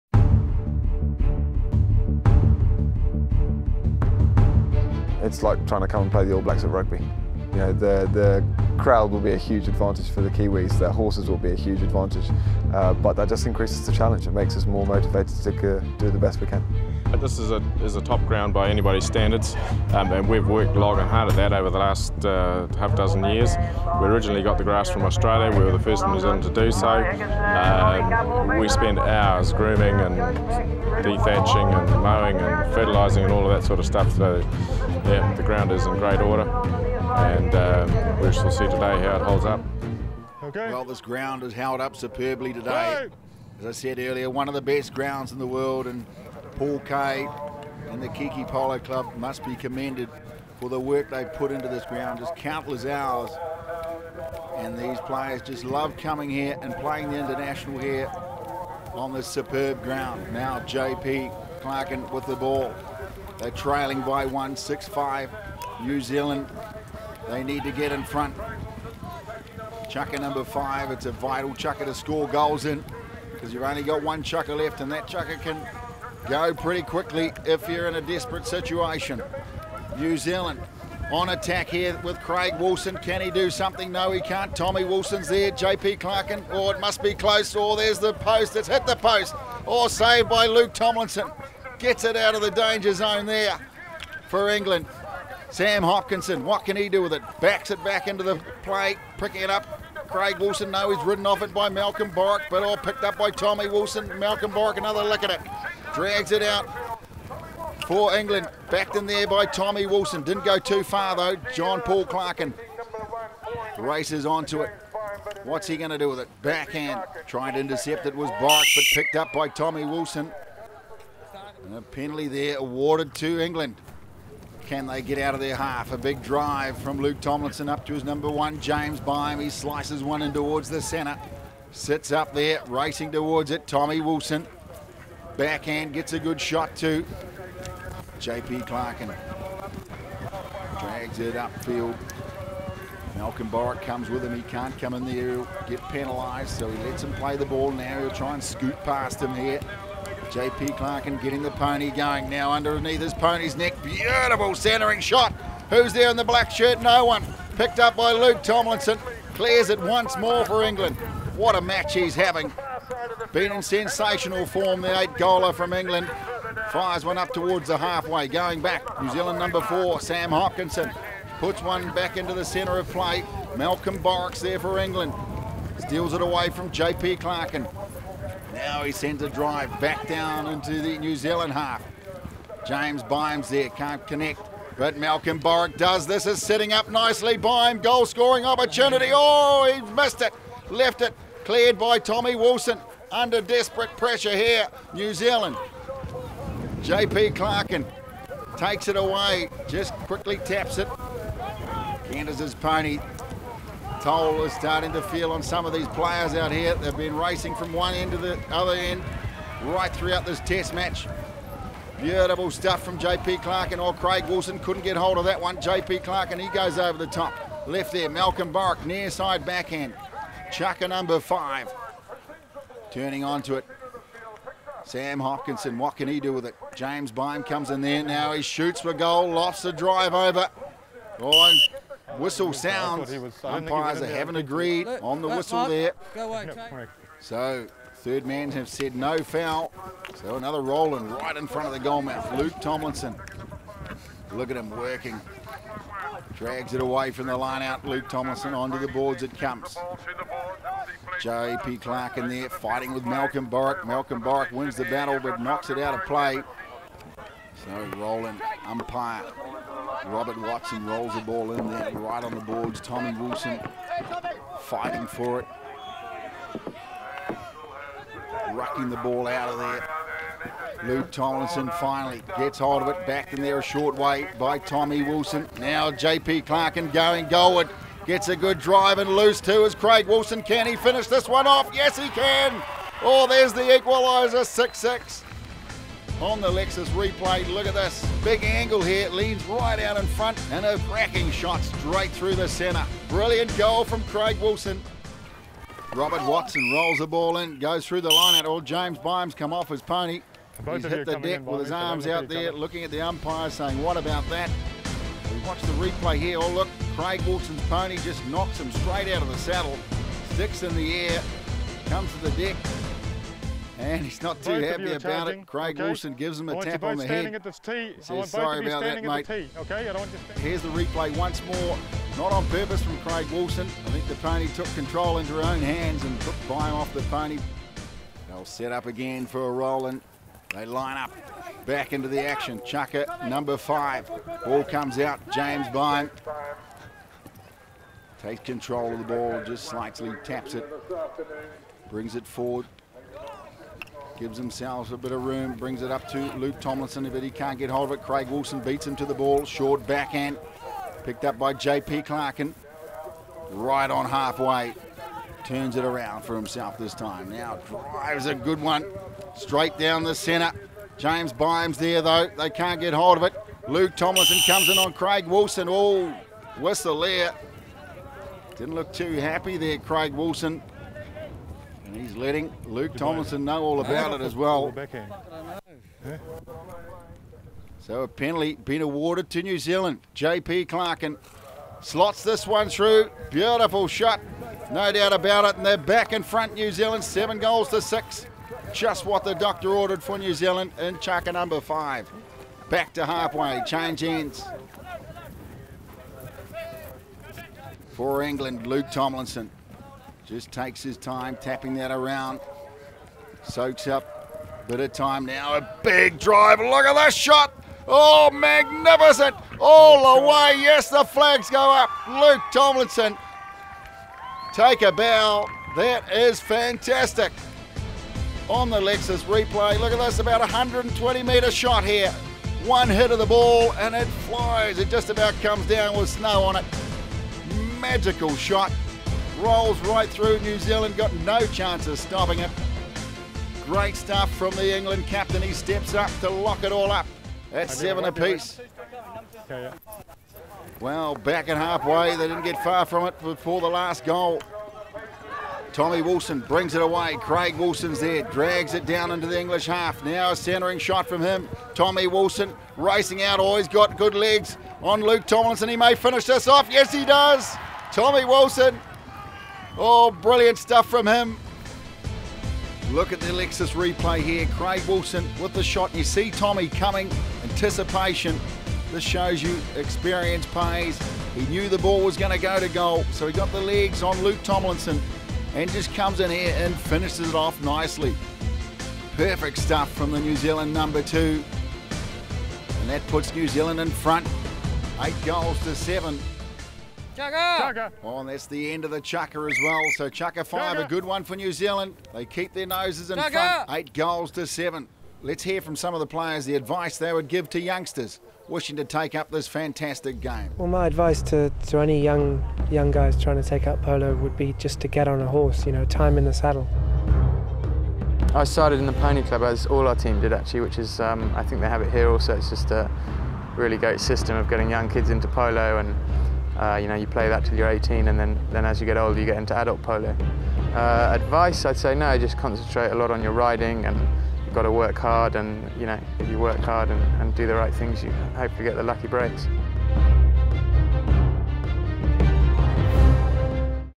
2010 Part 6 - International Test Match Polo - New Zealand vs England
New Zealand vs England, played on February 13 2010 at Kihikihi in the Waikato, New Zealand.

Part 6 - Chukka 4 and player interviews

New Zealand:
No. 1 Craig Wilson 6 goals
No. 2 Thomas Wilson 7 goals
No. 3 J.P. Clarkin 8 goals
No.4 Sam Hopkinson 6 goals Total 27 goals

England
No.1 James Beim 7 goals
No.2 Mark Tomlinson 7 goals
No.3 Malcolm Borwick 6 goals
No.4 Luke Tomlinson 8 goals Total 28 goals